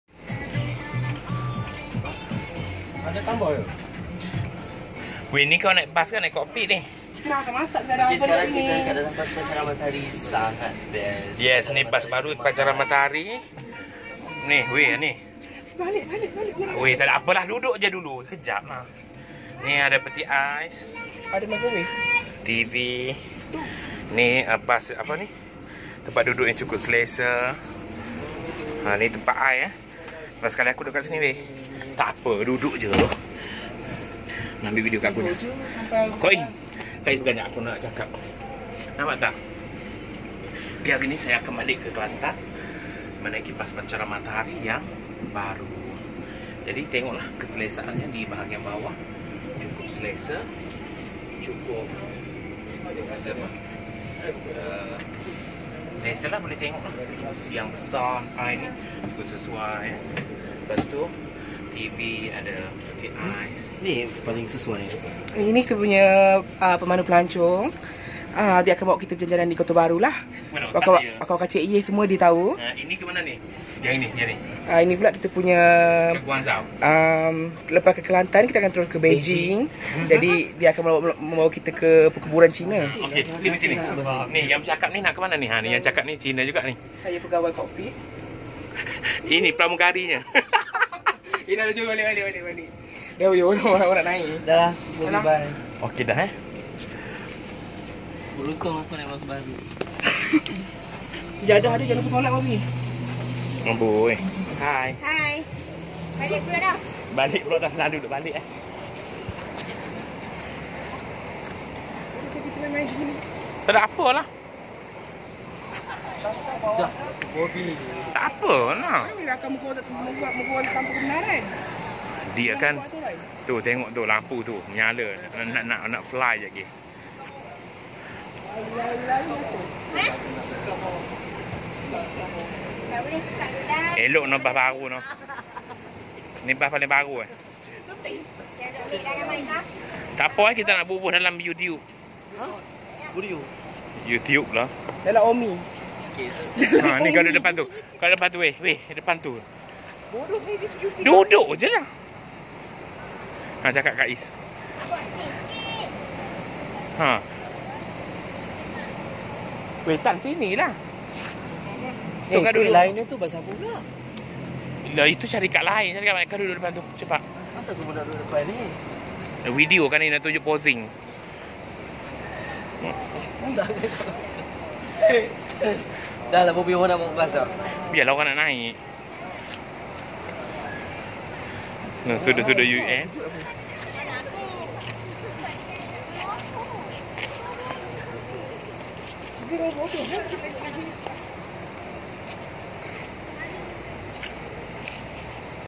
5 STAR - BAS PANCARAN MATAHARI DARI DALAM, PERKHIDMATAN TERBAIK
Perjalanan saya dari Changloon ke Kota Bharu menaiki bas pancara Matahari yang terbaru.  Inilah antara sebab saya akan selalu memilih Bas Pancaran Matahari kerana service juga cukup terbaik.  Anda semua boleh comment antara pengalaman anda bersama Pancran Matahari.